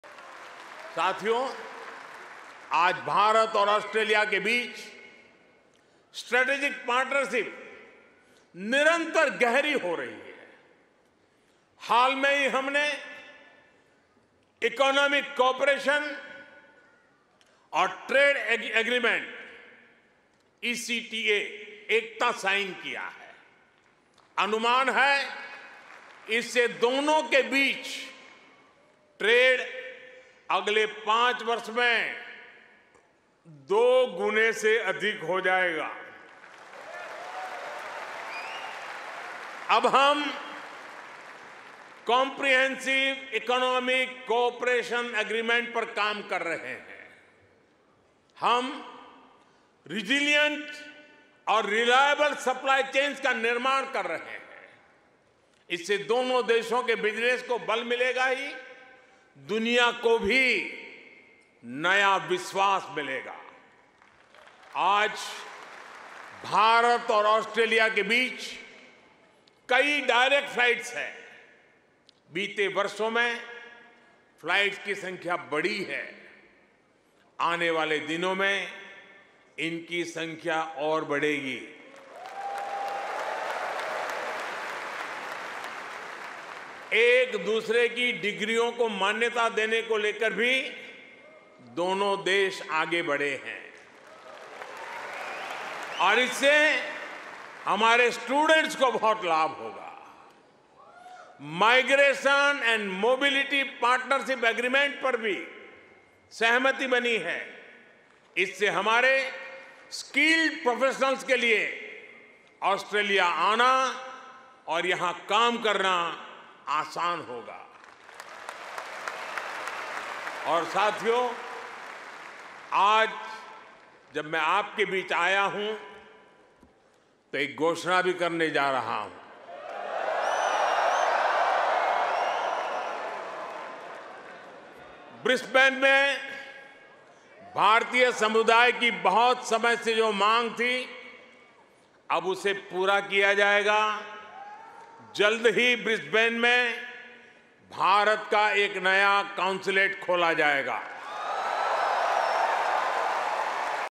Sydney में PM Modi की इस घोषणा से तालियों की गड़गड़ाहट से गूंज उठा पूरा स्टेडियम!
PM Modi attended mega community programme in Sydney, Australia. Australian PM Albenese also joined in for the event. He said, "The basis of historic ties between India and Australia is mutual trust and mutual respect."